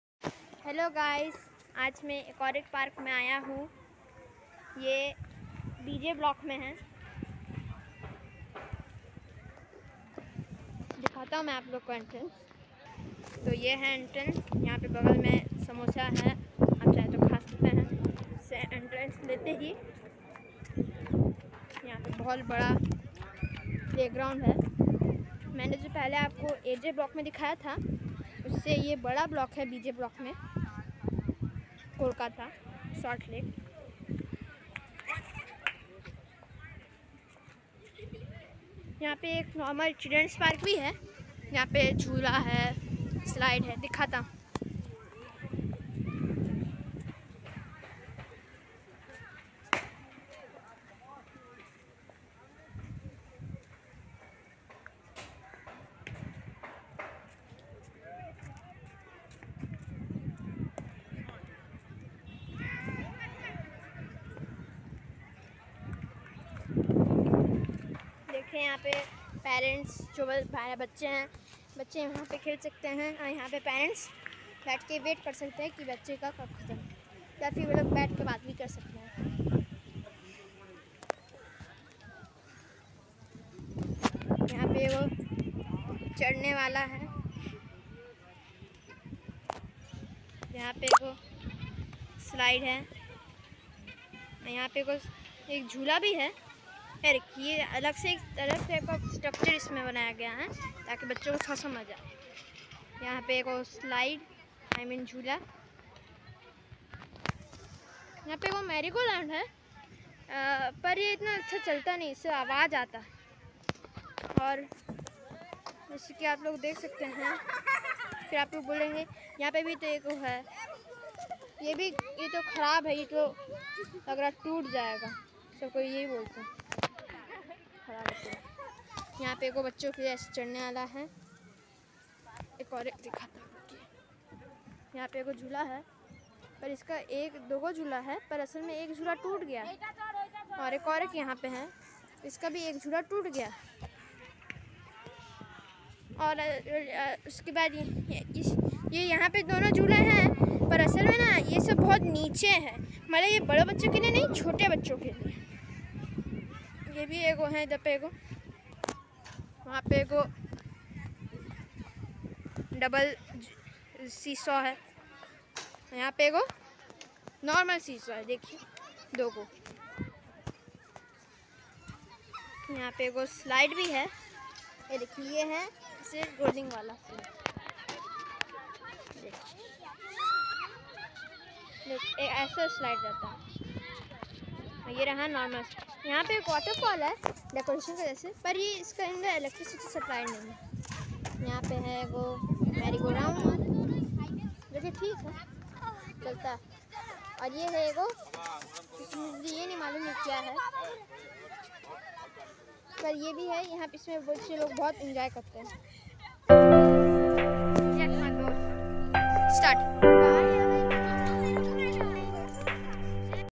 BJ block park visit
This park is located at salt lake BJ block kolkata